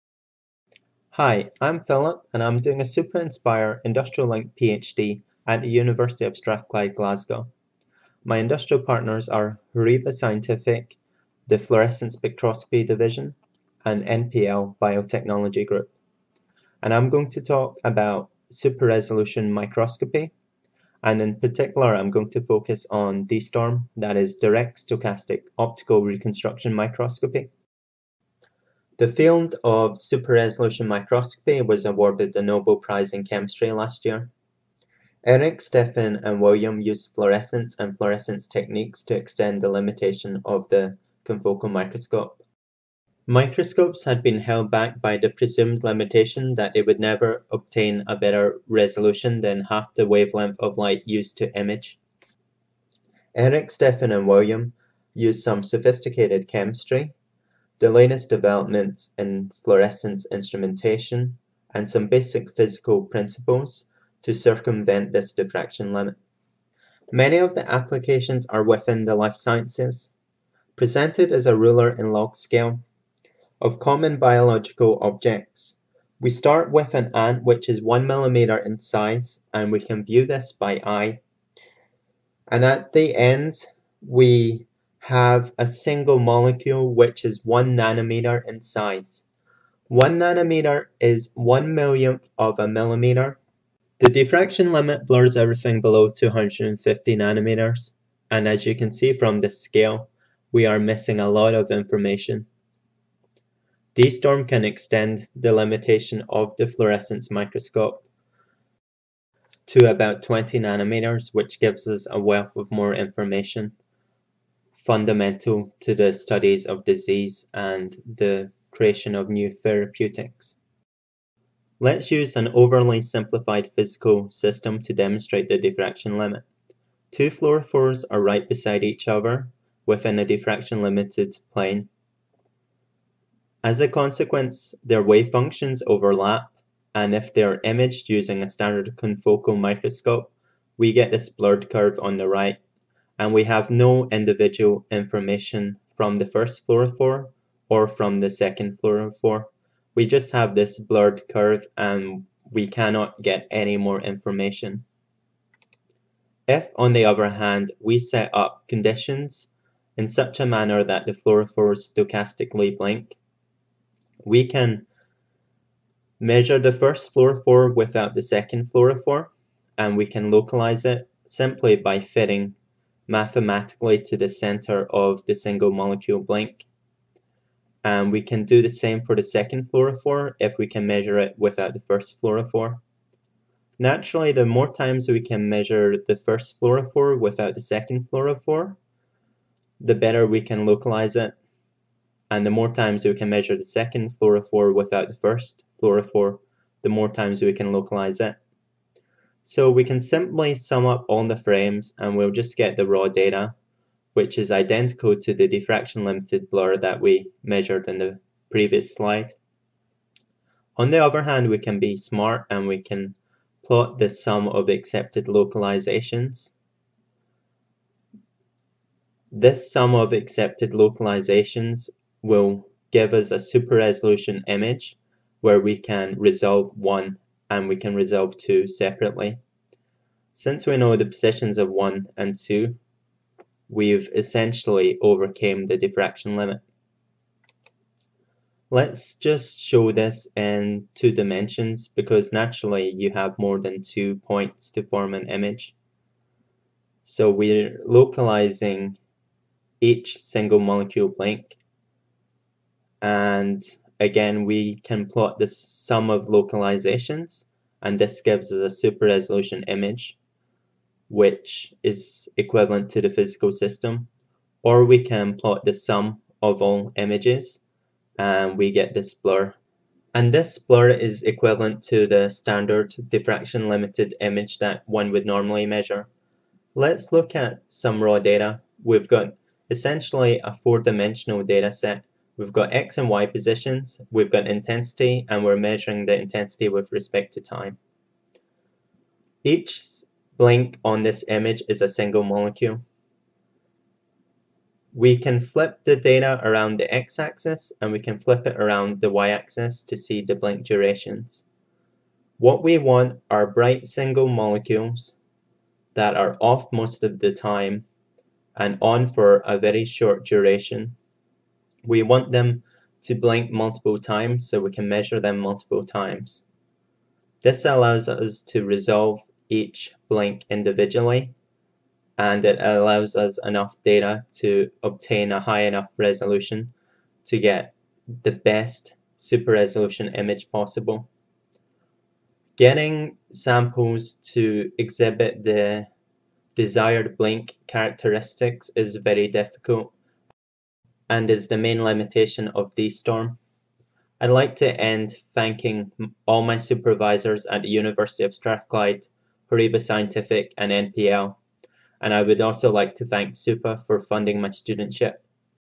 direct Stochastic Optical Reconstruction Microscopy (dSTORM)
A 6 minute video I made during my PhD. Basic principles behind dSTORM super-resolution microscopy.